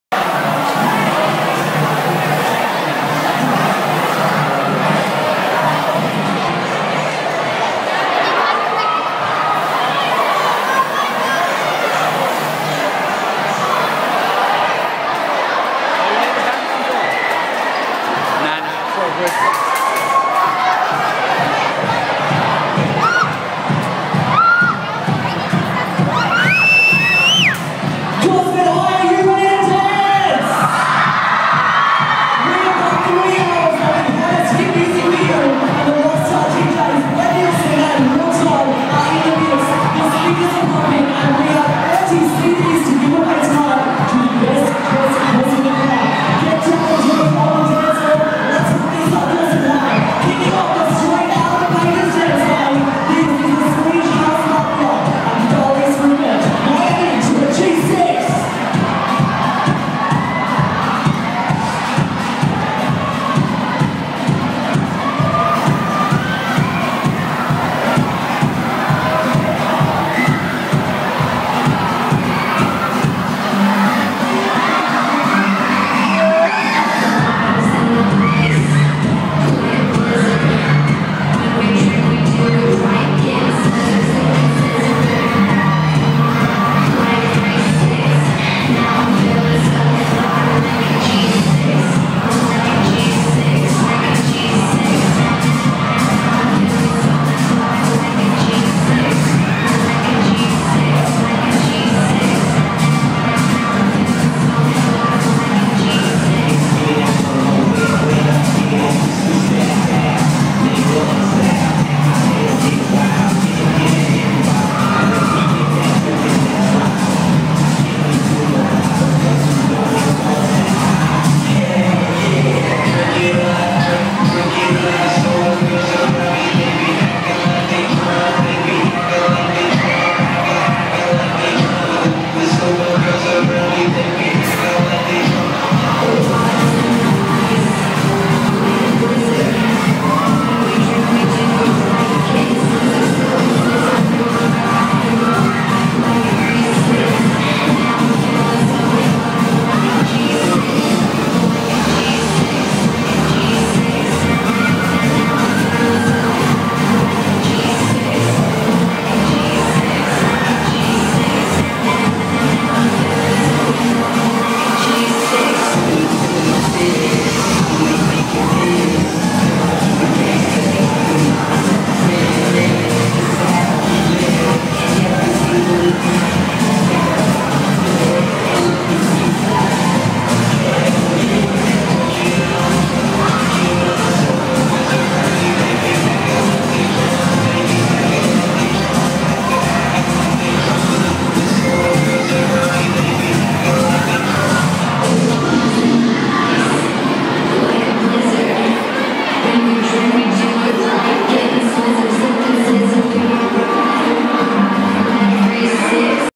Gosford High Term 1 Disco Opening
Opening Gosford High's term 1 disco - MC by Nic Kelly, lighting by Mat Kappert.